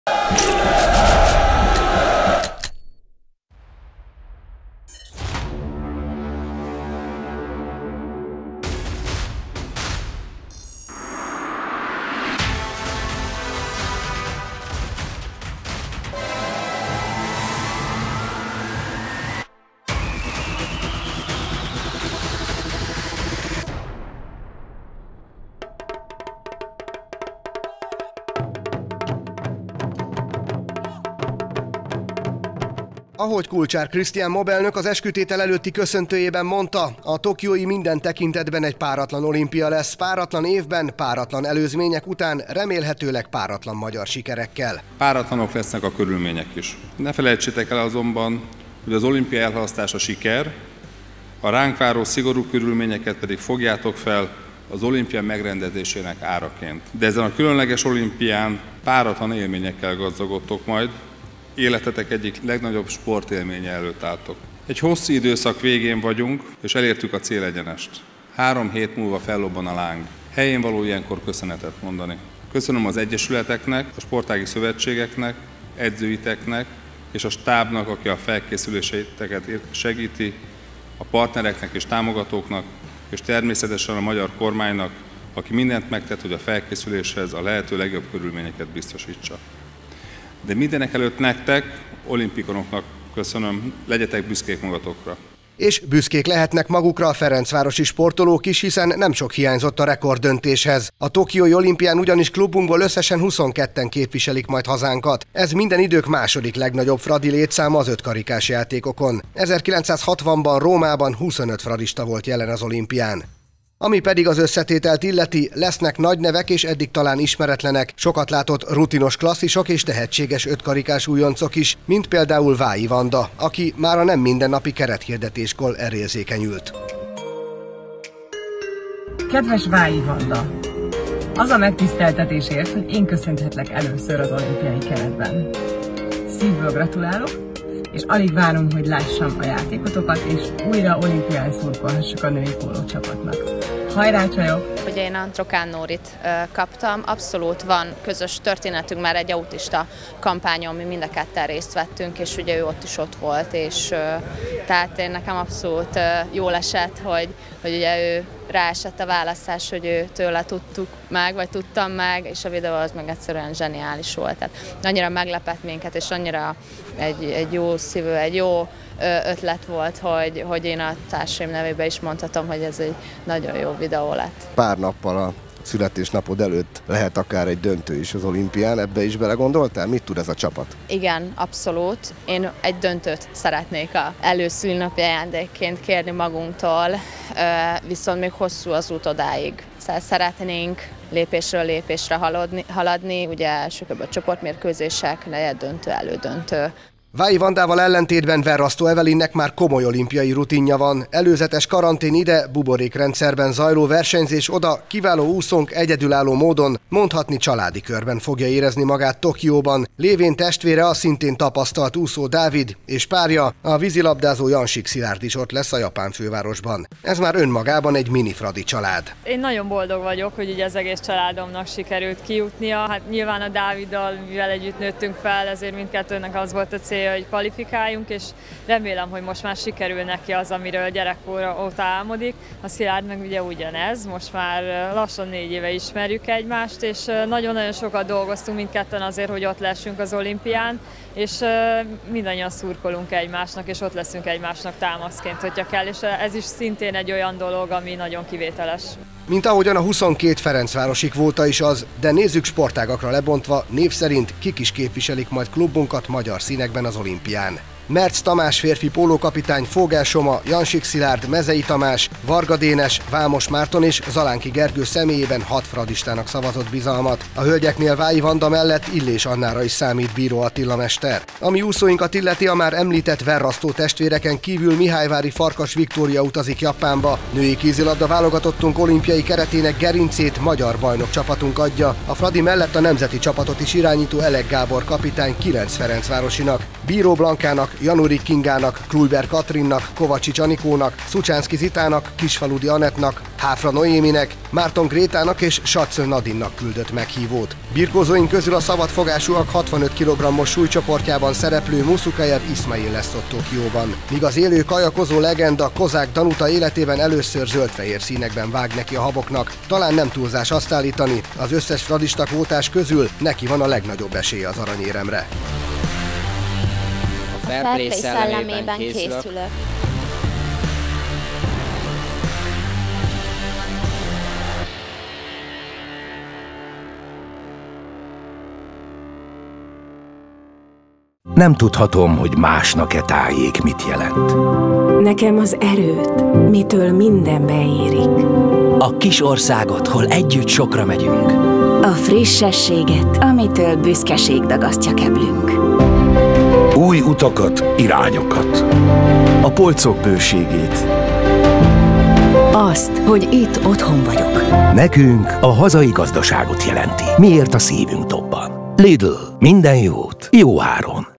FM | Olimpiai eskütétel | 2021.07.07.
Letették az esküt olimpiára készülő sportolóink. Tokióban összesen 22 ferencvárosi sportoló képviseli majd a magyar színeket, köztük az a Verrasztó Evelyn, aki egyedülálló módon, "családi körben" szerepel majd az ötkarikás játékokon.